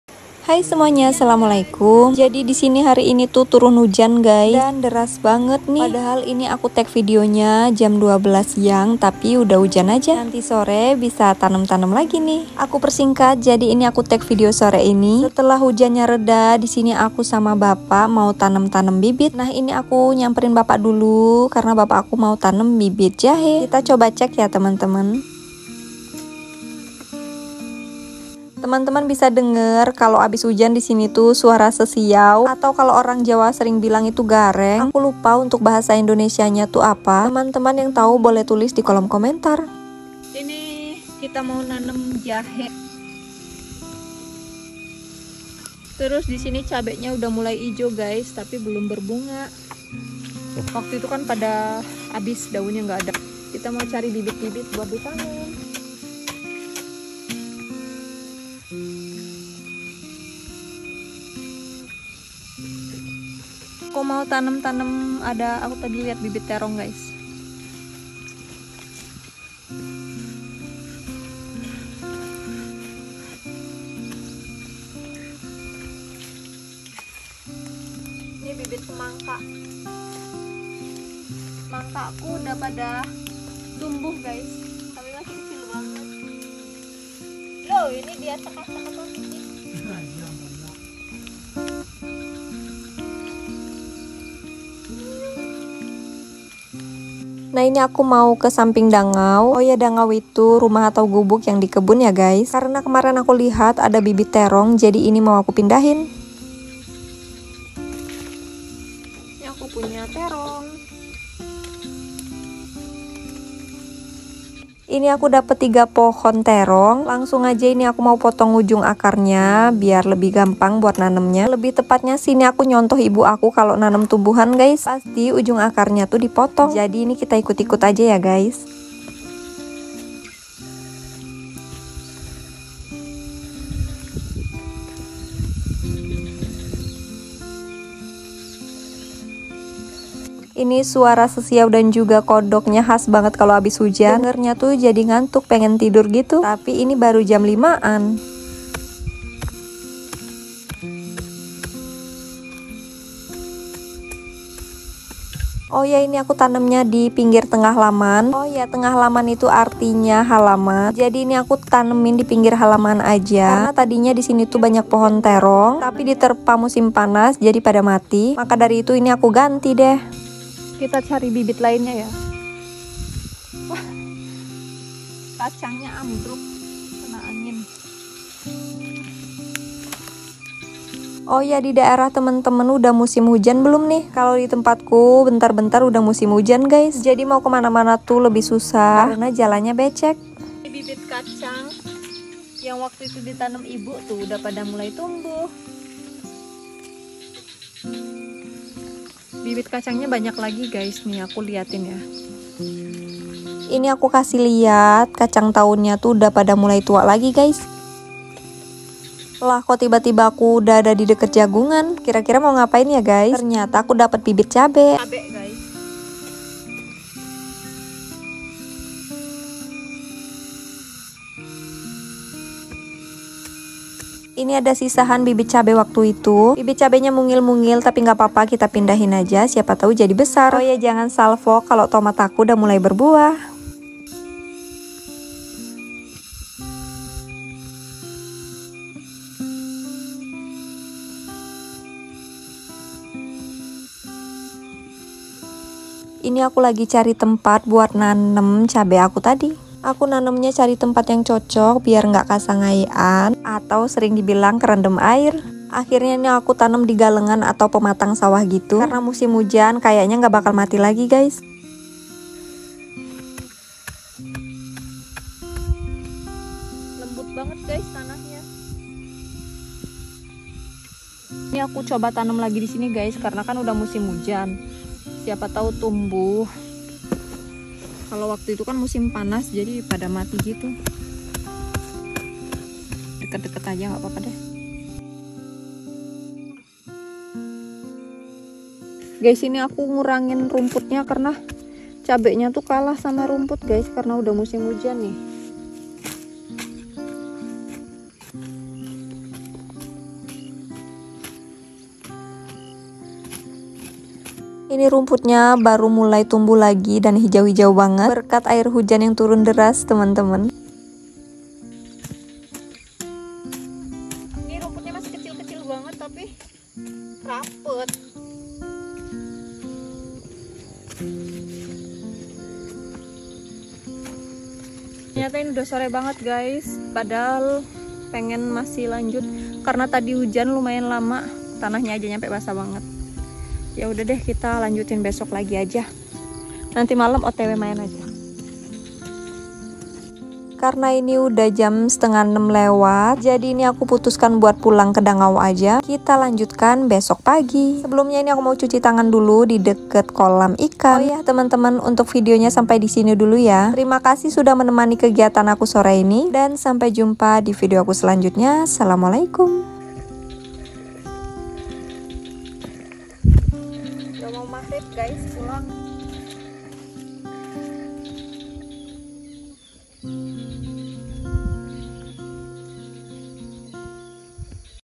Tanam sayuran di kebun
setelah hujjan reda, kami menanam sayuran di lahan yang sudah tersedia.